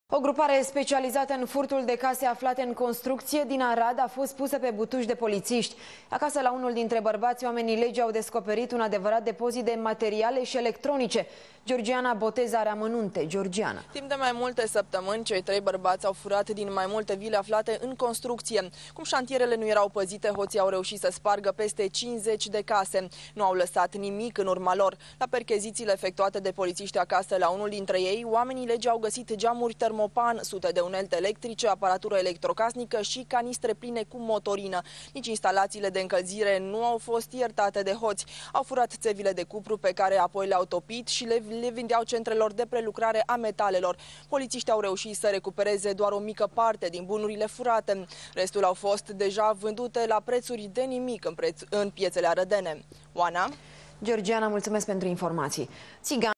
Au spart peste 50 de vile 3 FEBRUARIE 2011
O grupare specializata in furtul din case aflate in constructie din Arad a fost pusa pe butuci de politisti. Acasa la unul dintre barbati oamenii legii au descoperit un adevarat depozit de materiale si electronice.